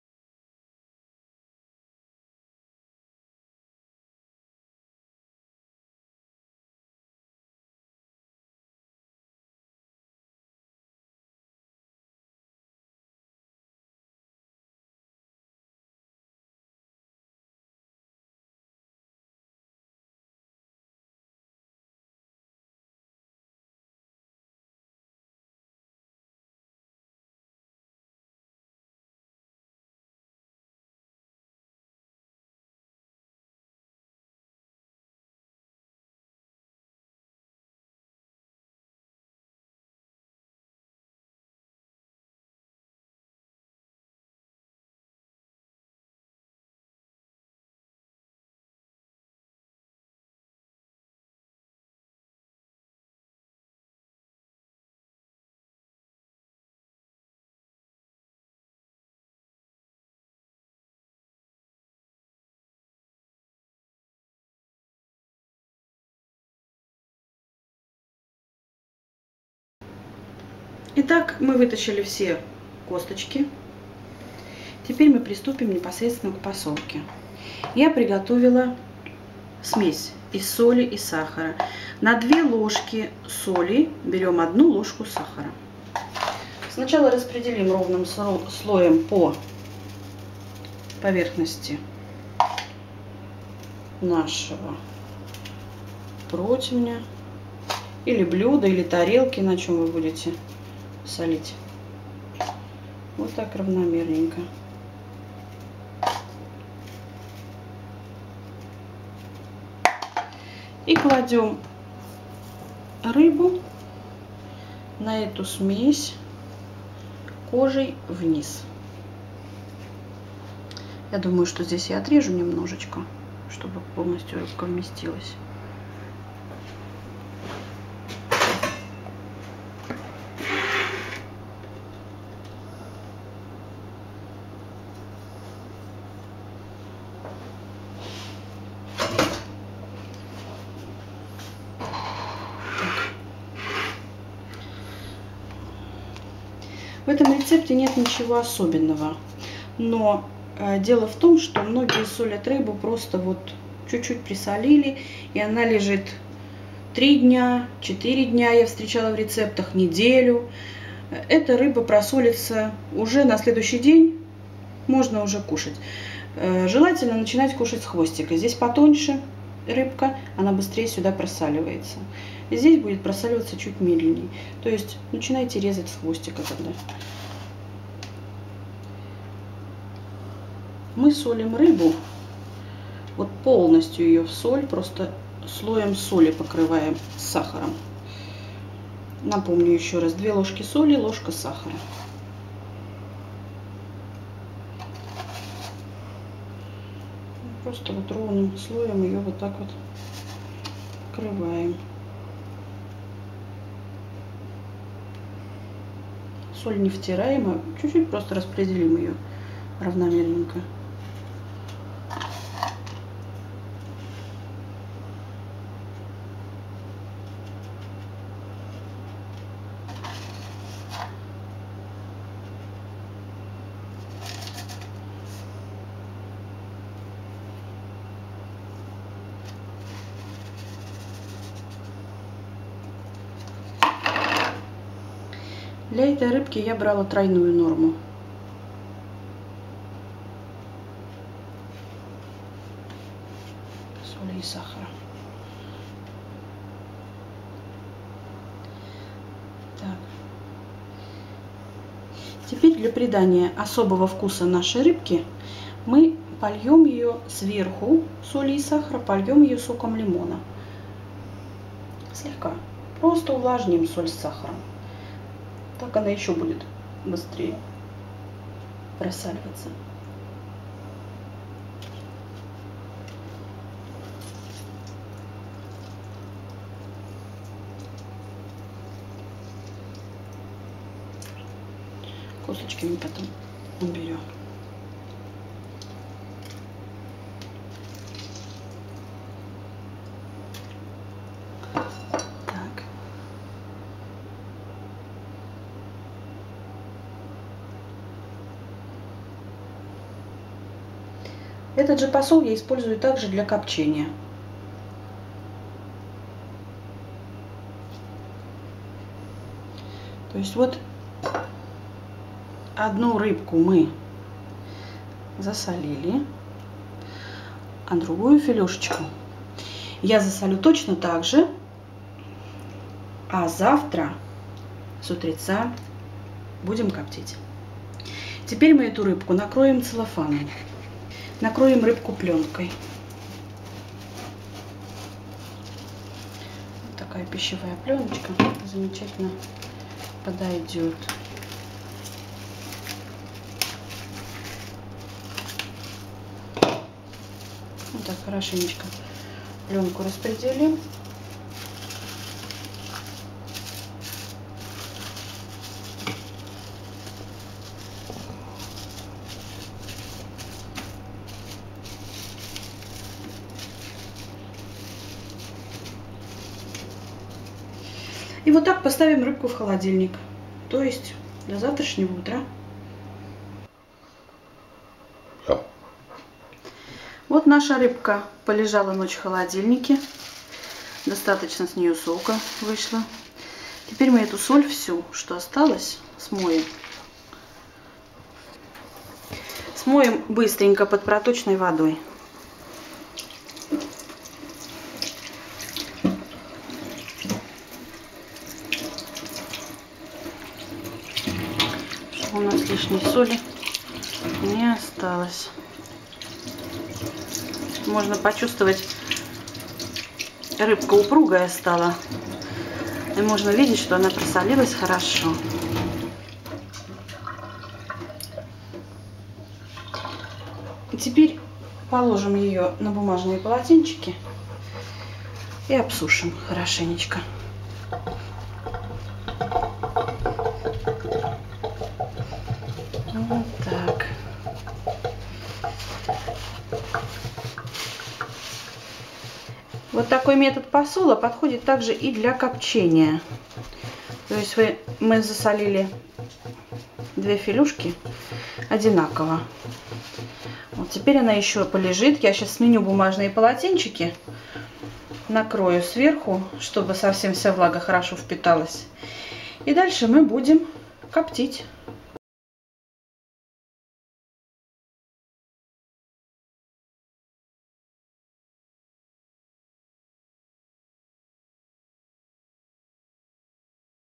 Засолка лосося (быстрый способ), часть 2.
Это серия из трёх видео, в которых я покажу как разделать лосось (или любую другую рыбу) на филе с кожей, второе видео о том, как засолить филе лосося (быстрый способ, при котором рыба готова уже на следующий день) и третье видео о том как закоптить лосось холодным способом (лосось холодного копчения).